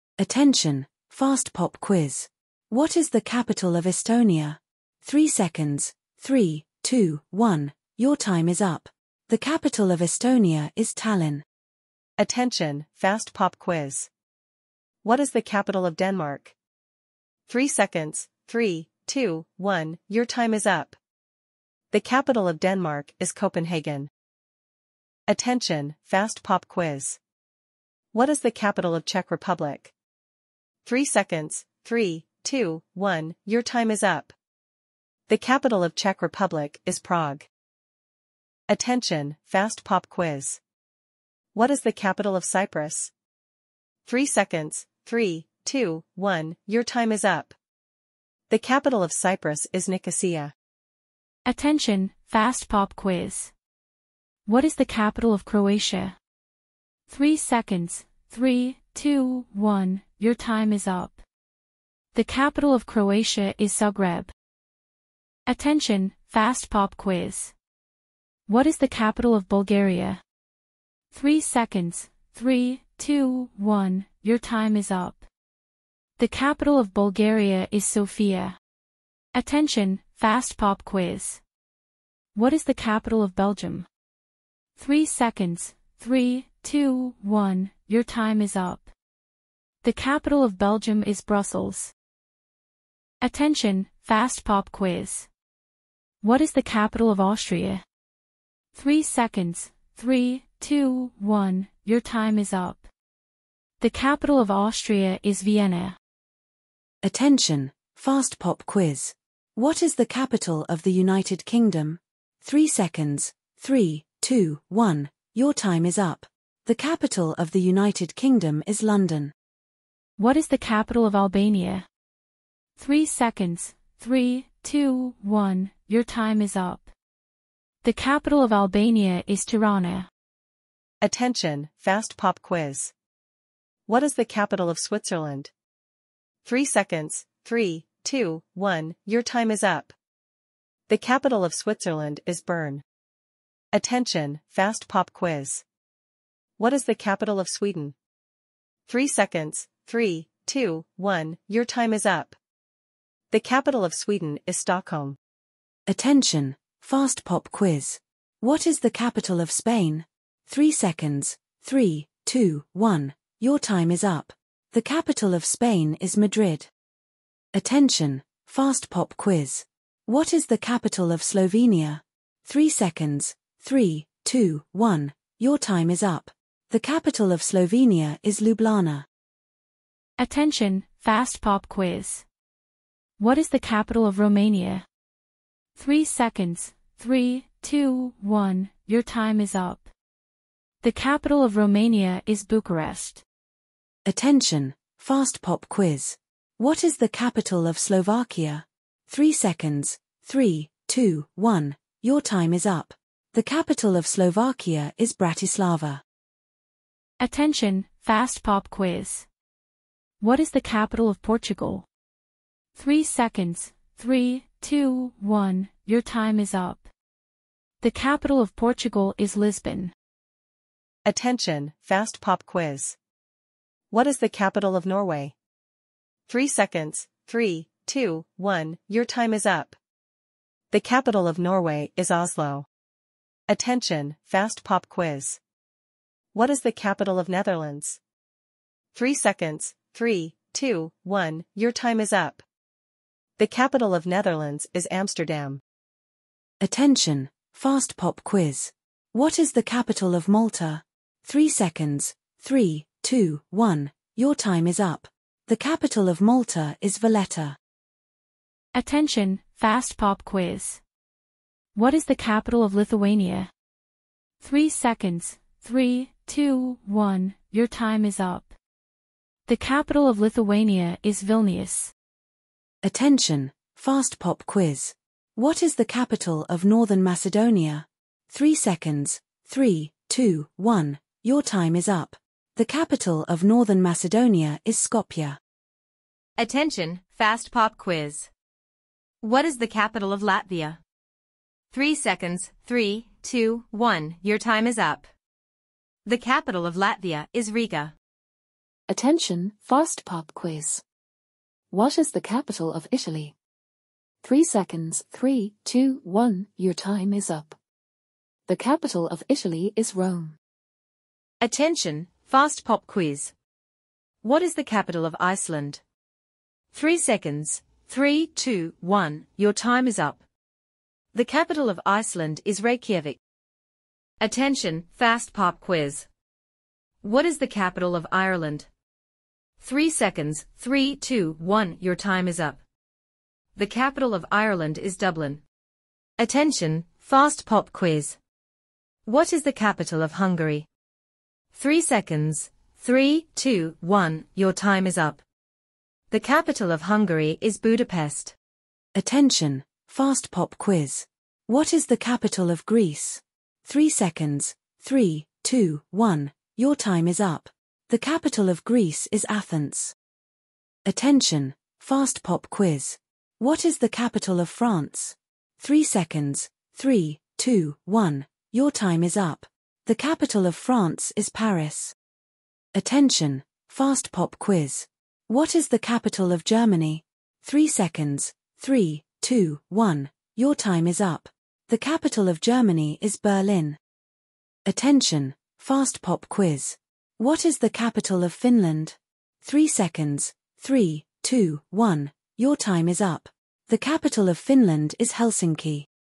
6 Minutes of European Capitals Quizzes for Listening 🌍🎥🎓
6 minutes geography pop quizzes on the capitals of European countries for listening.